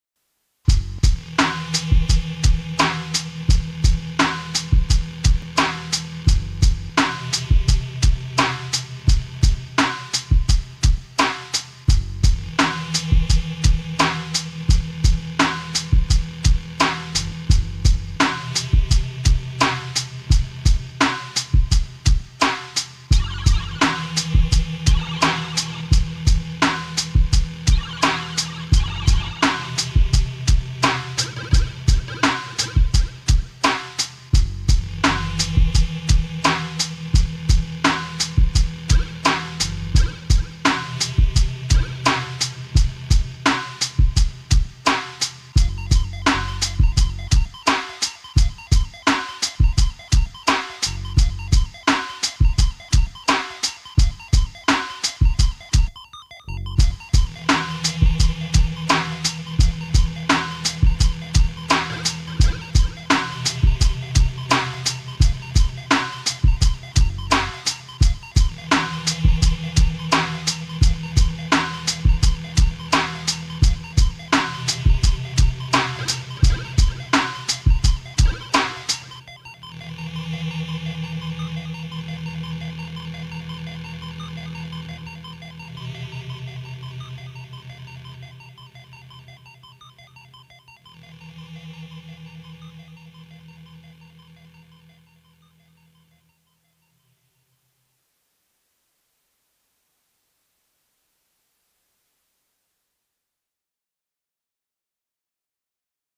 WeDoBeats 100 Beats Challenge // Beat 01 //
The goal is to make 100 beats in 100 days. Yeah i totally lost my mind lol. Anyway, here is my first beat.